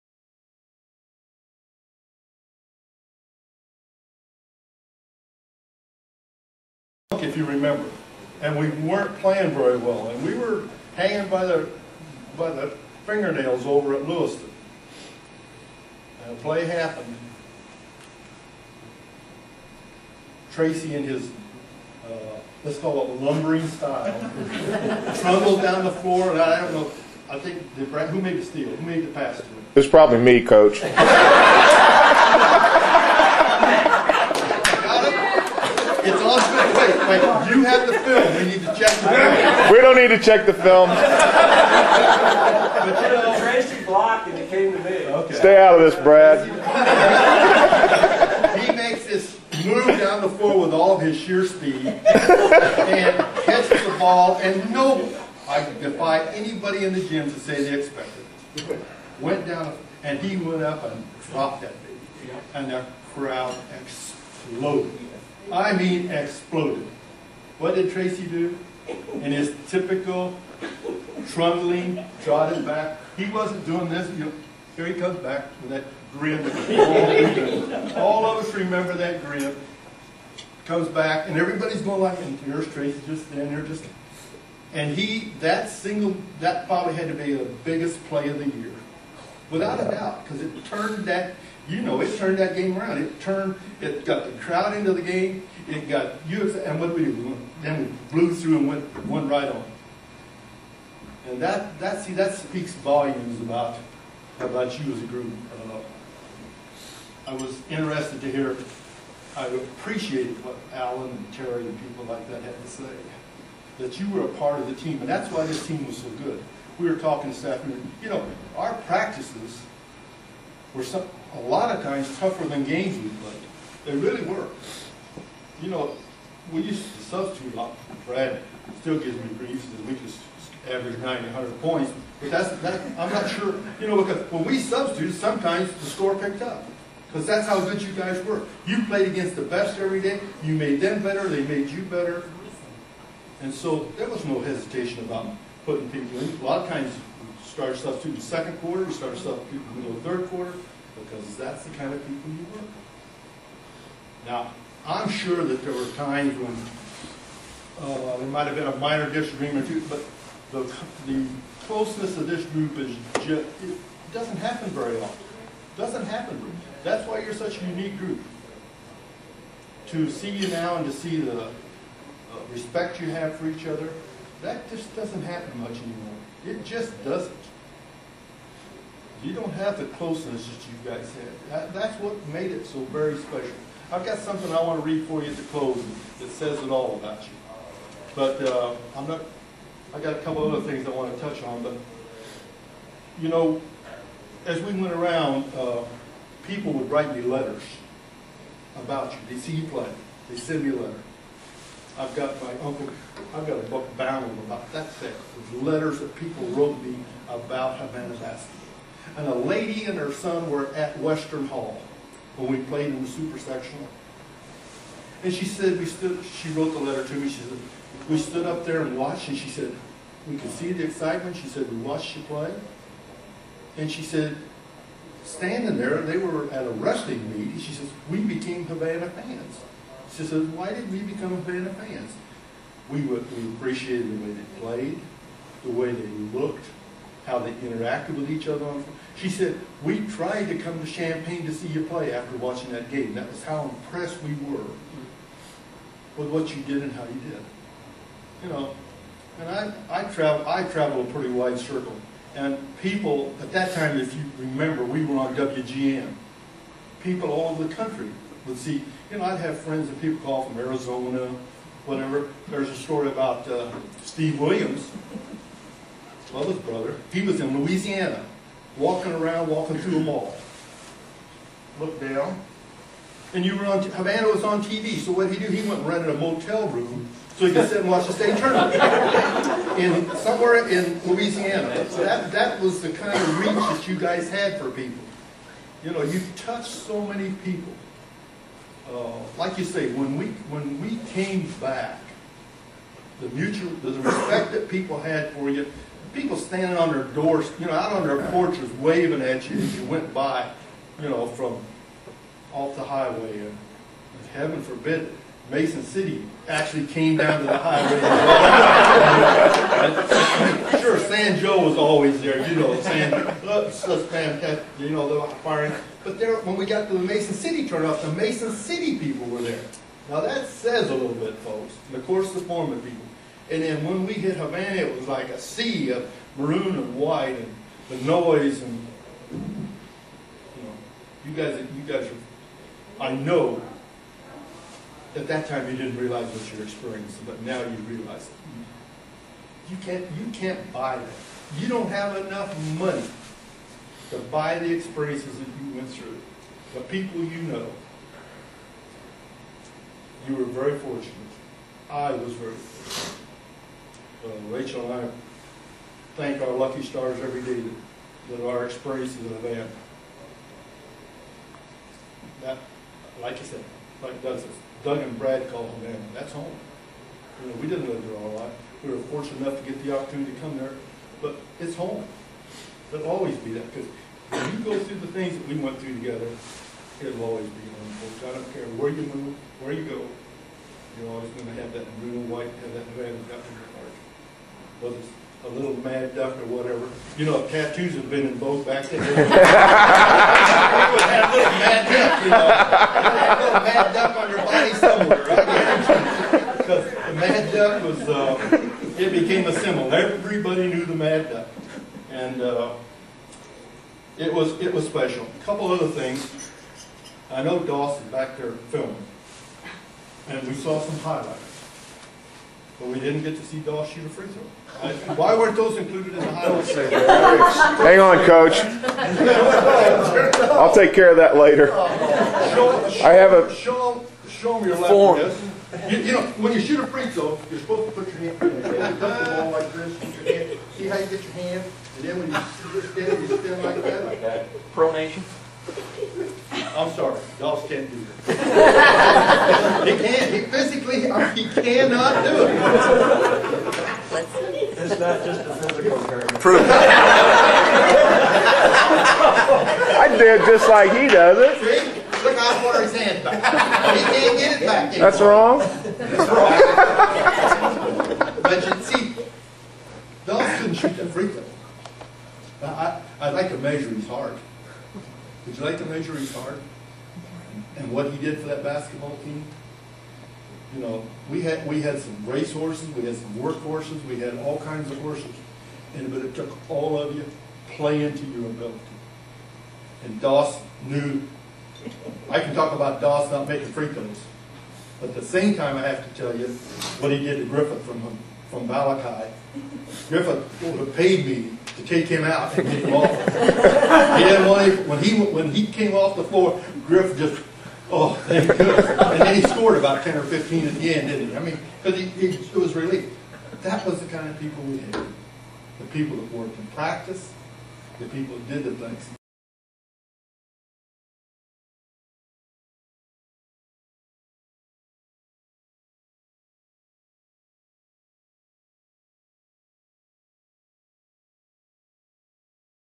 Coach Gregurich's Speech @ 30 Year Reunion Pt. 1
Coach Gregurich's speech to the 30 Year Reunion of the 1979 Havana Ducks State Tournament team.